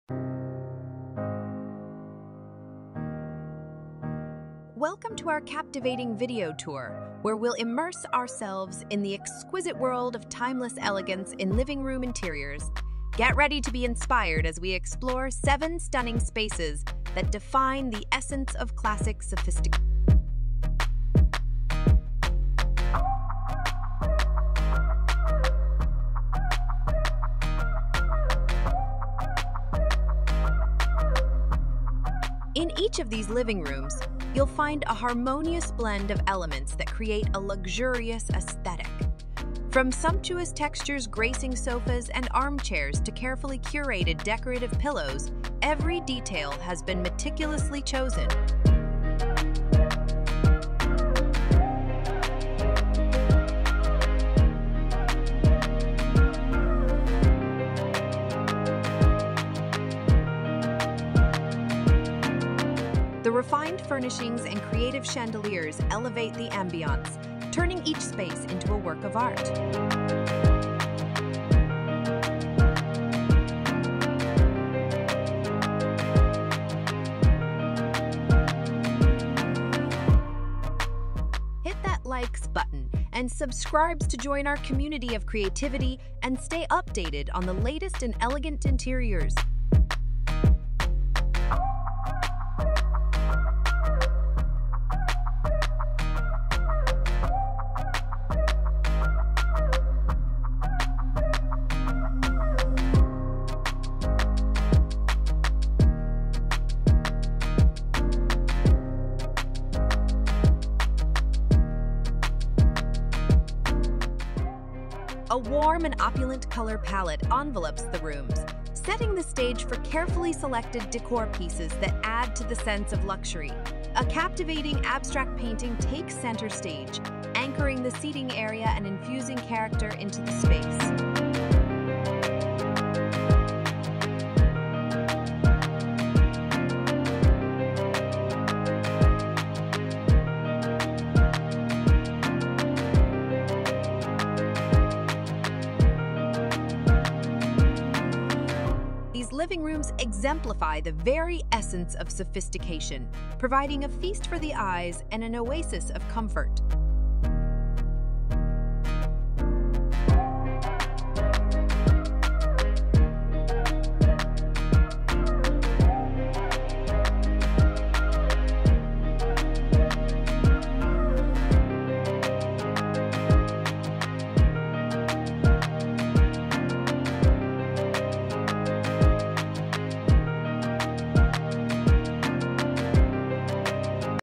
Beyond Trends: 7 Timeless Living Room Interior Design Ideas in Contemporary Classic American Style
🏠 Step into Timeless Elegance: Welcome to a captivating video tour where we embark on a journey through the exquisite realm of living room interiors that embody the essence of classic sophistication. Prepare to be captivated as we delve into 7 stunning spaces that define luxury and charm.

In each of these living rooms, a symphony of carefully orchestrated elements comes together to create a tapestry of opulent design. From the indulgent textures gracing sofas and armchairs to the meticulously curated decorative pillows, every aspect has been thoughtfully selected. The presence of refined furnishings and imaginative chandeliers elevates the ambiance, transforming each room into a true masterpiece.

🌆 A Palette of Opulence: As you step into these spaces, you'll be enveloped in a warm and opulent color palette that sets the tone for a lavish experience. Exquisitely chosen decor pieces are strategically placed, enhancing the feeling of luxury at every turn. A captivating abstract painting commands attention, serving as the heart of the seating area and infusing character into the entire room.

🛋️ Balancing Tradition and Modernity: What truly sets these living rooms apart is the seamless blend of timeless craftsmanship and contemporary allure. It's an atmosphere that celebrates the best of both worlds, offering an indulgent visual treat and an oasis of comfort.

🎨 A Celebration of Aesthetics: Join us on this captivating journey as we explore the harmony between classic design and modern sensibilities. These living rooms are a testament to the enduring appeal of aesthetics that transcend time. Feast your eyes on the symphony of elegance, and let comfort and luxury envelop you.

Don't miss the chance to experience the timeless allure of these living room interiors. Join us as we celebrate the beauty that stands the test of time and discover the captivating symphony of classic and contemporary design.

00:00 - 1
00:21 - 2
00:47 - 3
01:12 - 4
01:36 - 5
02:02 - 6
02:26 - 7